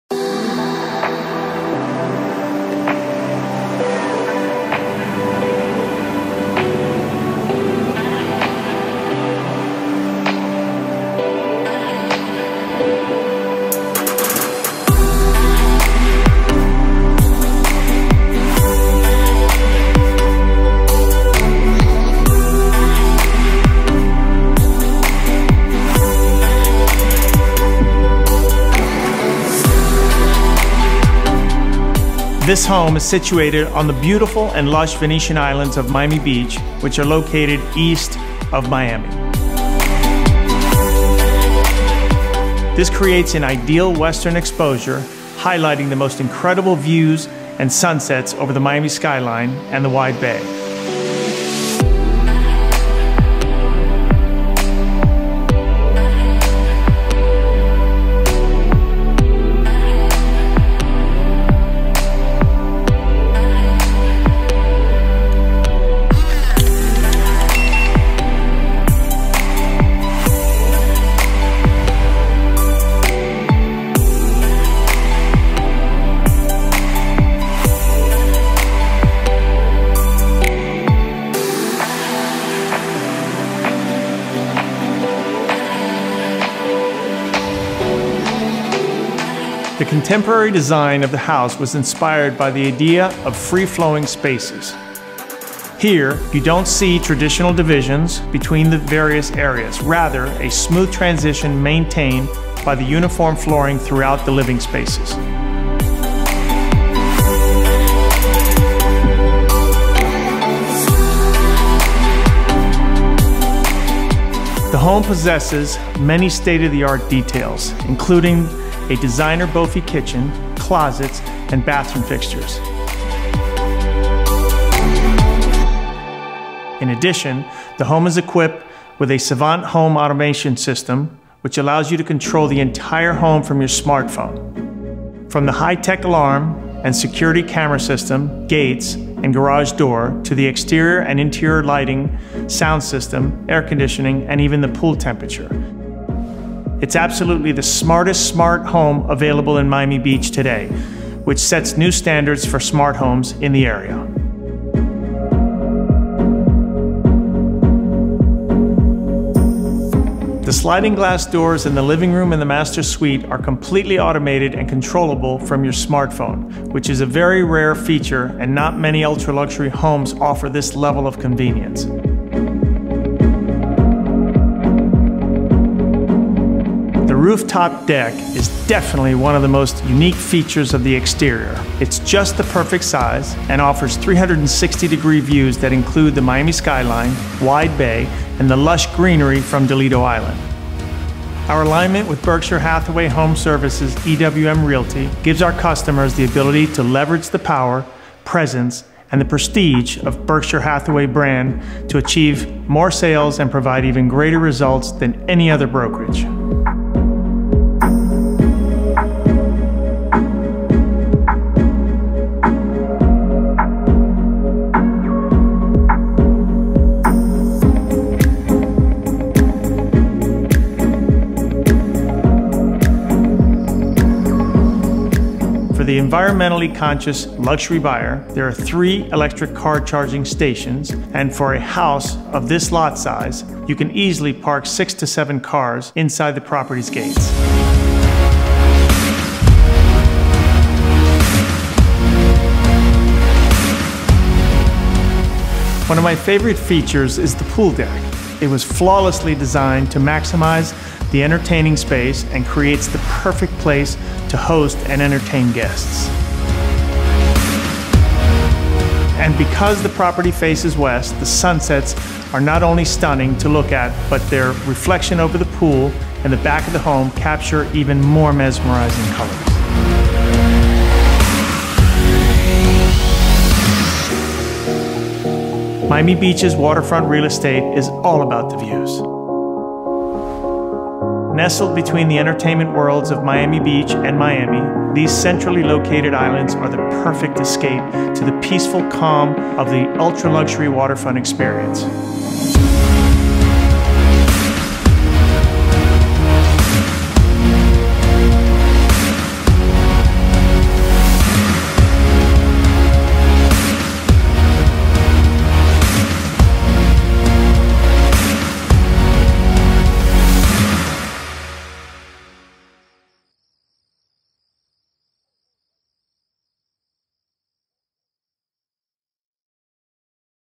SOLD: $12,500,000 - 500 Dilido Drive, Miami Beach | Experience Ultra Luxury With Nelson Gonzalez
The 8th sold home in 2019 with a selling price of over $10 Million for Top Producer Nelson Gonzalez at BHHS EWM Realty.

If you are still looking for a new waterfront home equivalent to the quality of 500 W Dilido Drive, then 10 W San Marino Drive may be perfect for you.

Berkshire Hathaway HomeServices EWM Realty invites you to discover what ultra-luxury home in Miami Beach looks like at 500 W Dilido Drive. A modern waterfront masterpiece equipped with the latest smart technology, facing west, and offering incredible sunsets over Biscayne Bay and Miami skyline.

If you need help selling or buying,
Contact Nelson Gonzalez